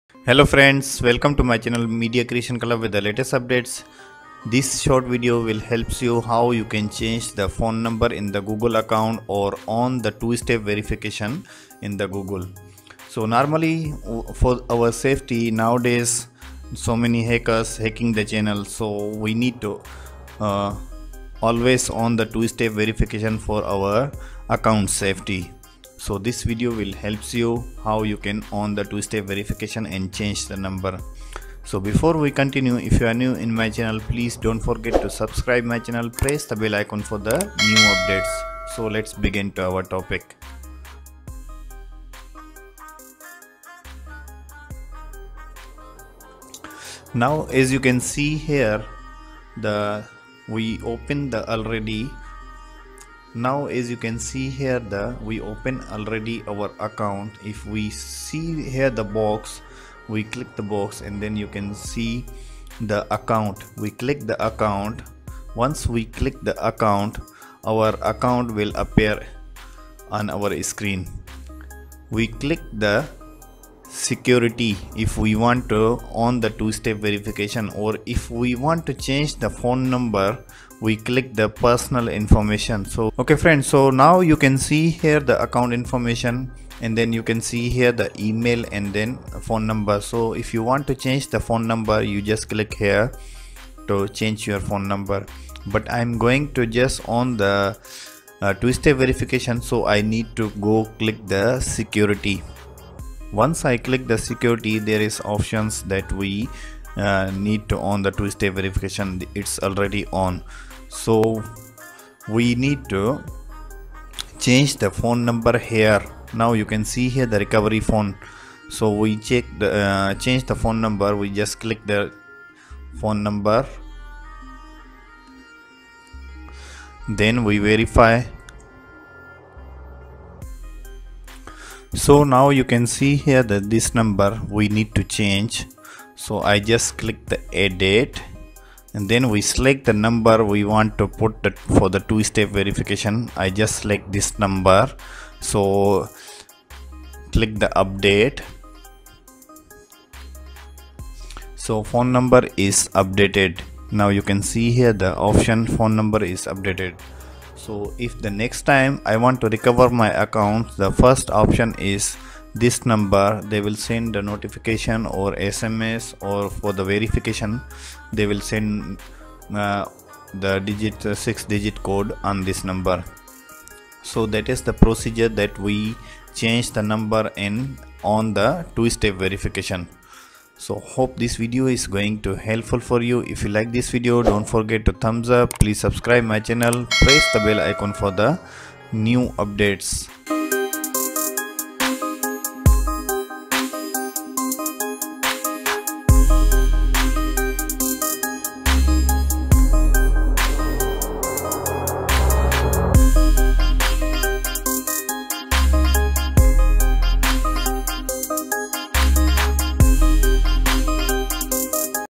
How To Change Recovery Phone Number And Turn On Two-Step Verification On Google 2022
This Video Media Creation Club Providing You How To Change Recovery Phone Number And Turn On Two-Step Verification On Google 2022

Like ____🌷Share_____💞Comment

عزیز ناظرین
شکریہ